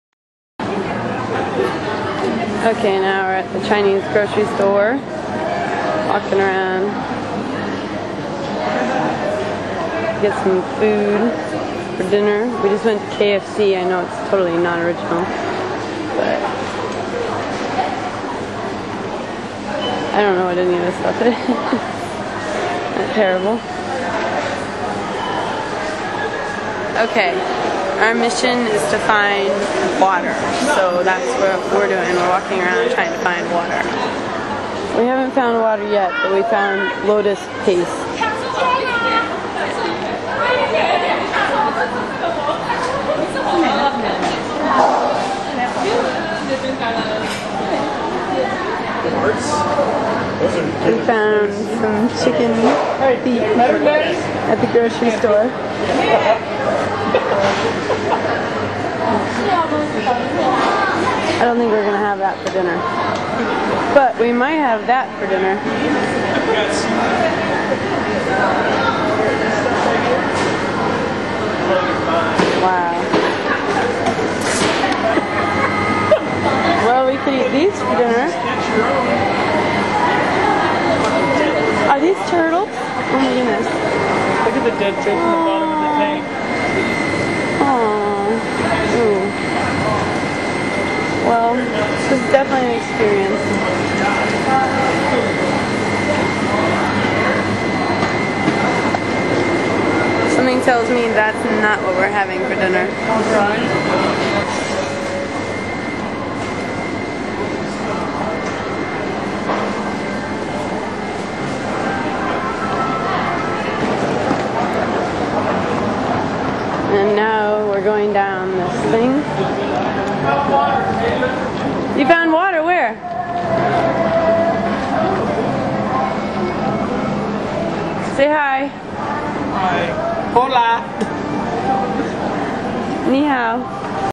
9/26/06 Shanghai World Cup Webcam #2 - Chinese Grocery Store
This is a video of a Chinese Grocery Store.